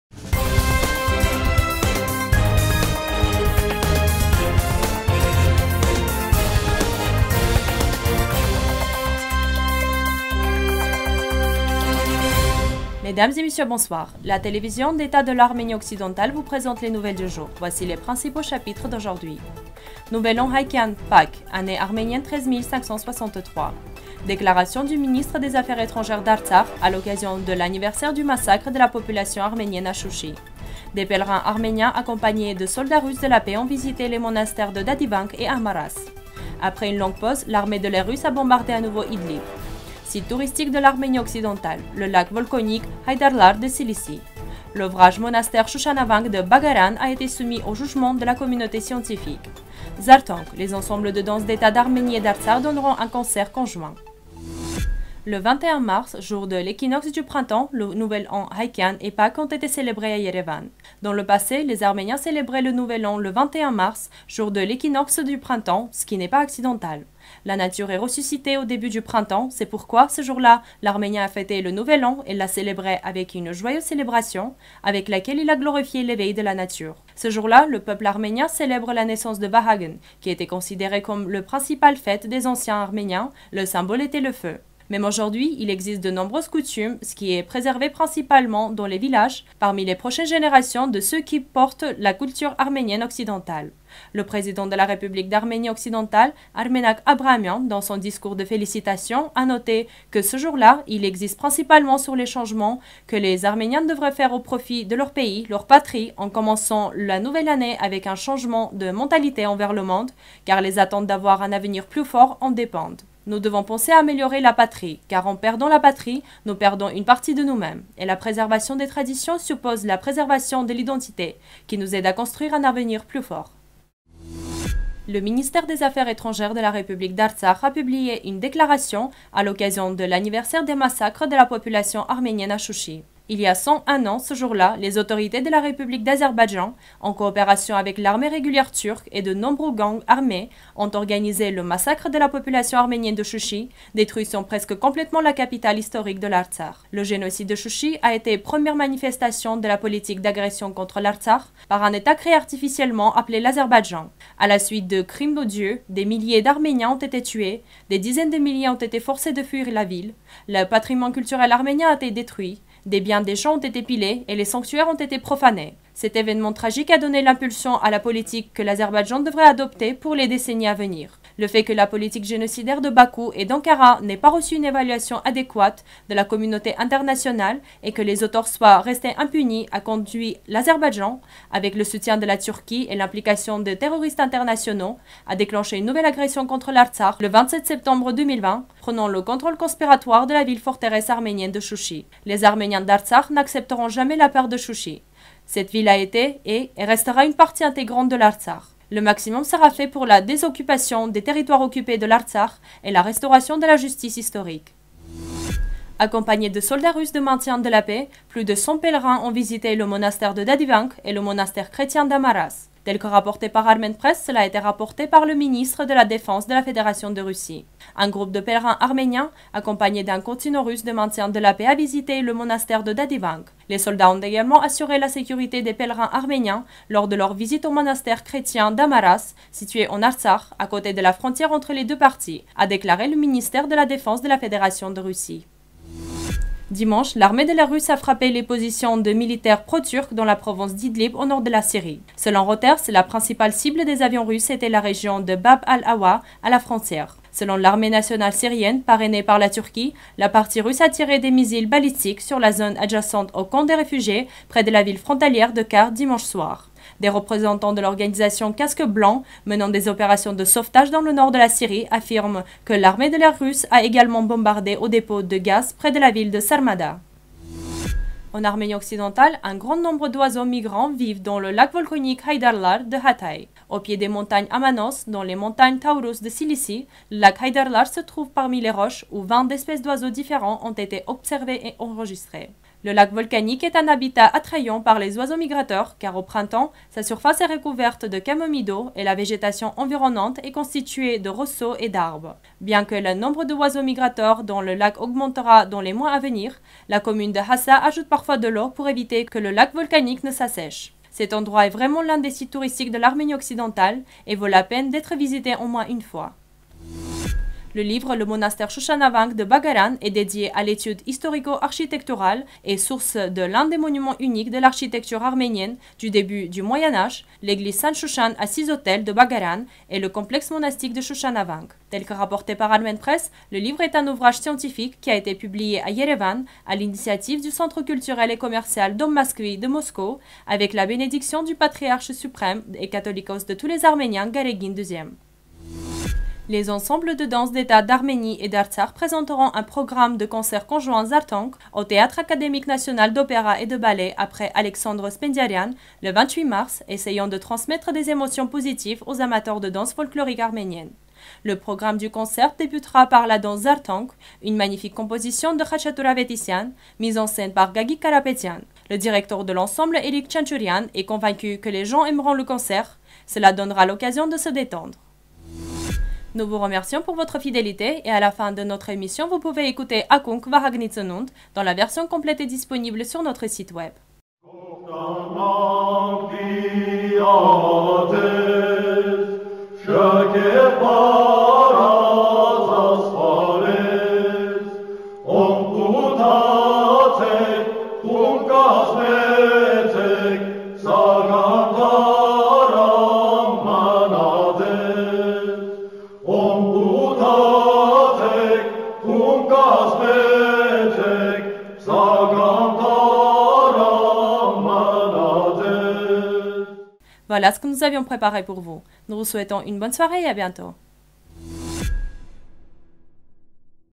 Nouvel An Haykian - Pâques. Année arménienne 13563 .Nouvelles 22.03.2021
1. Nouvel An Haykian - Pâques. Année arménienne 13563
2. Déclaration du MAE d'Artsakh à l'occasion de l'anniversaire du massacre de la population arménienne à Chouchi
3. Des pèlerins arméniens accompagnés de soldats russes de la paix ont visité les monastères de Dadivank et Amaras
4. Après une longue pause, l'armée de l'air russe a bombardé à nouveau Idlib
5. Sites touristiques de l'Arménie Occidentale. Le lac volcanique Haydarlar de Cilicie
6. L'ouvrage "Monastère Shoushanavank de Bagaran" a été soumis au jugement de la communauté scientifique
7. «Zartonk». Les ensembles de danse d'État d'Arménie et d'Artsakh donneront un concert conjoint